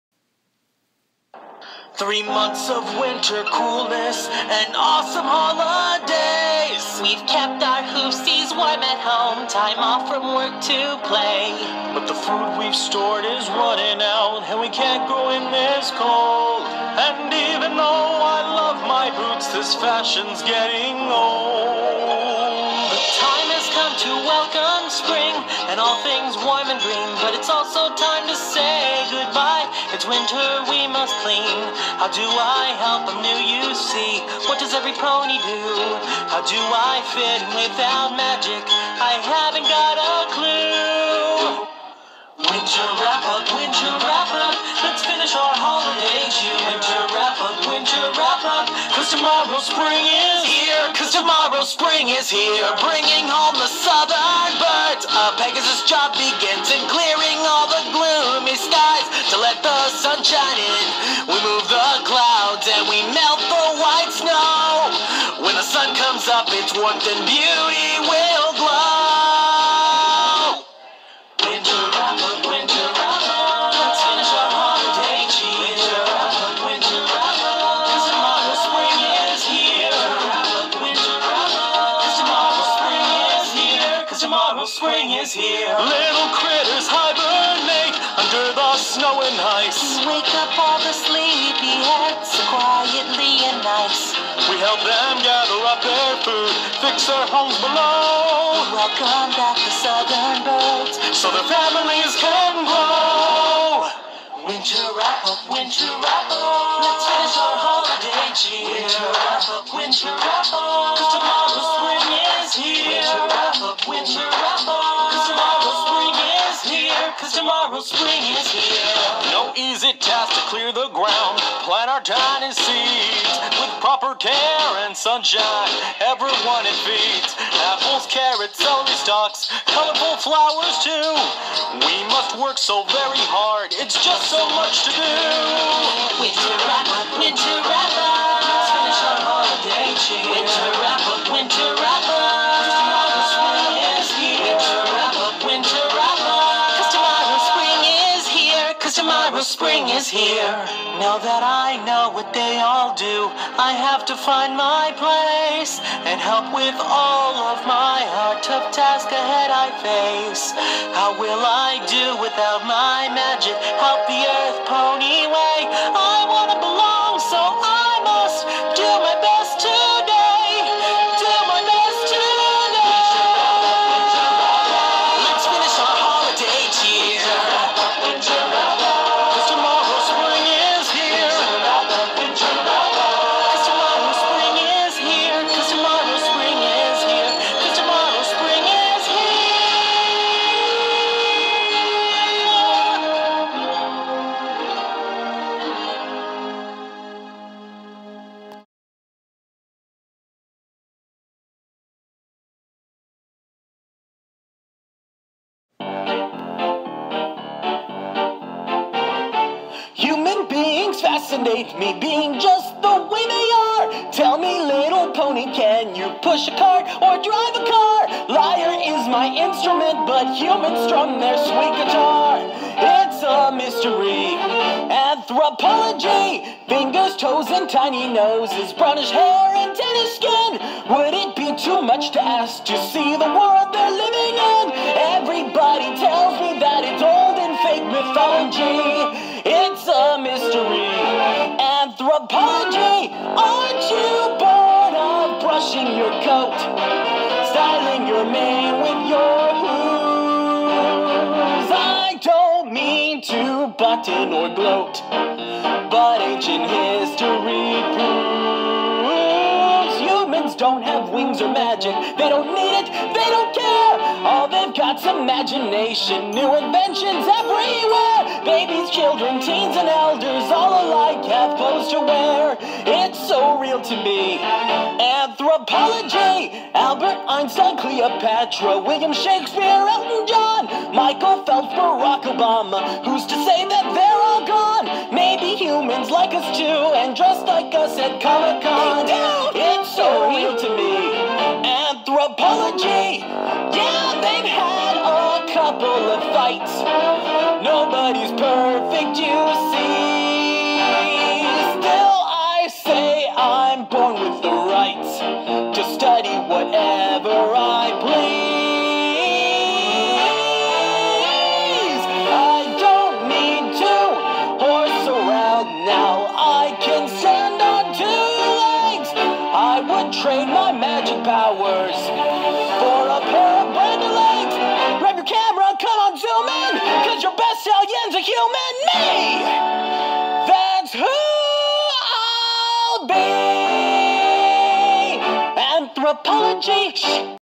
MLP:Fim Colt Version
Something different for today, I was looking at all the MLP characters in an opposite gender. So I decided to make a video about most of them. I'm pretty sure I got everyone that everyone wanted to see.